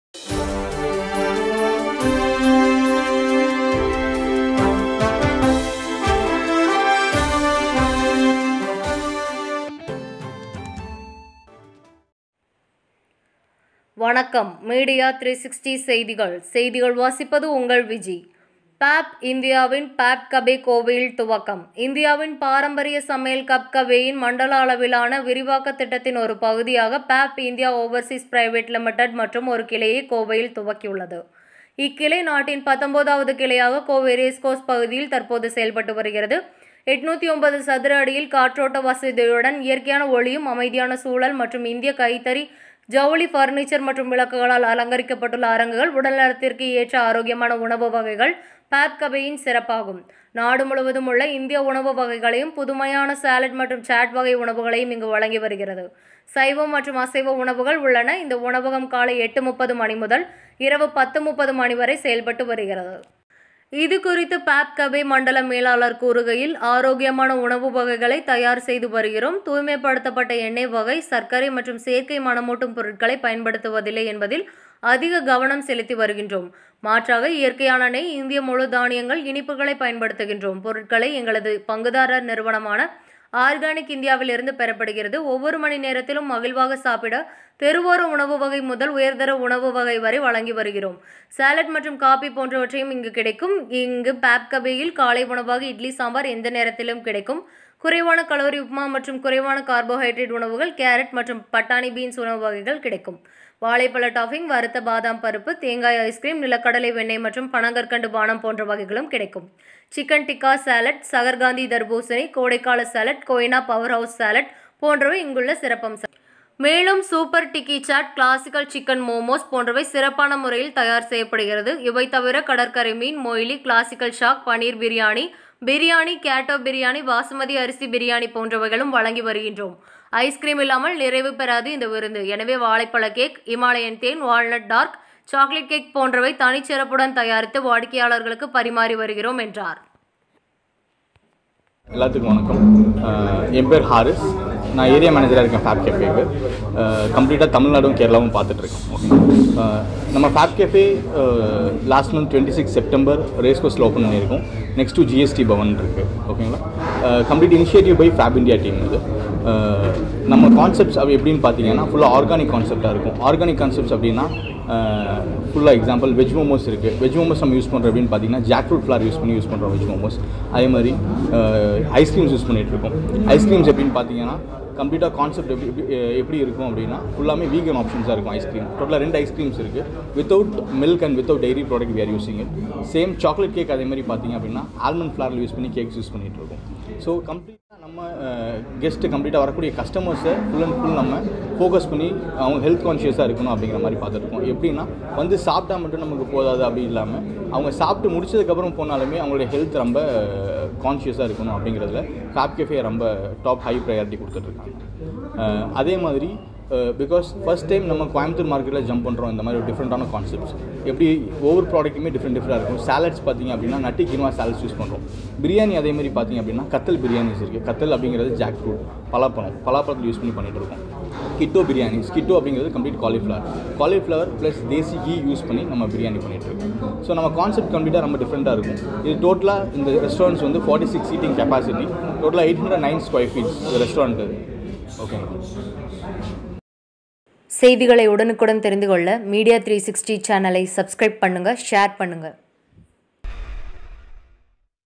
ஃபேப் இந்தியாவின் ஃபேப் கபே கோவையில் துவக்கம்
இந்தியாவின் பாரம்பரிய சமையல் கப் கபே-ன் மண்டல அளவிலான விரிவாக்கத் திட்டத்தின் ஒரு பகுதியாக பேப் இந்தியா ஓவர்சீஸ் பிரைவேட் லிமிடெட் மற்றும் ஒரு கிளையை கோவையில் துவக்கியுள்ளது. இக்கிளை நாட்டின் 19ஆவது கிளையாக கோவை ரேஸ்கோர்ஸ் பகுதியில் தற்போது செயல்பட்டு வருகிறது. 809 சதுர அடியில் காற்றோட்ட வசதியுடன் இயற்கையான ஒளியும் அமைதியான சூழல் மற்றும் இந்திய கைத்தறி ஜவுளி பர்னிச்சர் மற்றும் விளக்குகளால் அலங்கரிக்கப்பட்டுள்ள அரங்குகள் உடல் நலத்திற்கு ஏற்ற ஆரோக்கியமான உணவு வகைகள் ஃபேப் கபே இன் சிறப்பாகும். நாடு முழுவதும் உள்ள இந்திய உணவு வகைகளையும் புதுமையான சாலட் மற்றும் சாட் வகை உணவுகளை இங்கு வழங்கி வருகிறது. சைவம் மற்றும் அசைவ உணவுகள் உள்ளன. இந்த உணவகம் காலை 8:30 மணி முதல் இரவு 10:30 மணி வரை செயல்பட்டு வருகிறது.

இதுகுறித்து பேப் கபே மண்டல மேலாளர் கூறுகையில், ஆரோக்கியமான உணவு வகைகளை தயார் செய்து வருகிறோம். தூய்மைப்படுத்தப்பட்ட எண்ணெய் வகை சர்க்கரை மற்றும் சேர்க்கை மணமூட்டும் பொருட்களை பயன்படுத்துவதில்லை என்பதில் அதிக கவனம் செலுத்தி வருகின்றோம். மாற்றாக இயற்கையான நெய் இந்திய முழுதானியங்கள் இனிப்புகளை பயன்படுத்துகின்றோம்.பொருள்களை எங்களது பங்குதாரர் நிறுவனமான ஆர்கானிக் இந்தியாவில் இருந்து பெறப்படுகிறது. ஒவ்வொரு மணி நேரத்திலும் மகிழ்வாக சாப்பிட தெருவோர உணவு வகை முதல் உயர்தர உணவு வகை வரை வழங்கி வருகிறோம். சாலட் மற்றும் காபி போன்றவற்றையும் இங்கு கிடைக்கும். பேப் கபேயில் காலை உணவாக இட்லி சாம்பார் இந்த நேரத்திலும் கிடைக்கும். குறைவான கலோரி உப்புமா மற்றும் குறைவான கார்போஹைட்ரேட் உணவுகள் கேரட் மற்றும் பட்டாணி பீன்ஸ் உணவு வகைகள் கிடைக்கும். வாழைப்பழ டாப்பிங்,  வறுத்த பாதாம் பருப்பு, தேங்காய் ஐஸ்கிரீம், நிலக்கடலை வெண்ணைய் மற்றும் பனங்கற்கண்டு பானம் போன்ற வகைகளும் கிடைக்கும். சிக்கன் டிக்கா சாலட், சகர் காந்தி தர்பூசணி கோடைக்கால சாலட், கொய்னா பவர் ஹவுஸ் சாலட் போன்றவை இங்குள்ள சிறப்பம்சங்கள்.

மேலும் சூப்பர் டிக்கி  சாட், கிளாசிகல் சிக்கன்  மோமோஸ்,  போன்றவை சிறப்பான முறையில் தயார் செய்யப்படுகிறது.  இவைதவிர கடற்கரை மீன் மொய்லி,  கிளாசிக்கல் ஷாக் பனீர் பிரியாணி,  பிரியாணி, கேட்டோ பிரியாணி, பாசுமதி அரிசி பிரியாணி போன்றவைகளும் வழங்கி வருகின்றோம்.  ஐஸ்கிரீம் இல்லாமல் நிறைவு பெறாது இந்த விருந்து. எனவே வாழைப்பழ கேக்,  இமாலயன் தேன், வால்நட் டார்ட், சாக்லேட் கேக் போன்றவை தனிச்சிறப்புடன் தயாரித்து வாடிக்கையாளர்களுக்கு பரிமாறி வருகிறோம் என்றார்.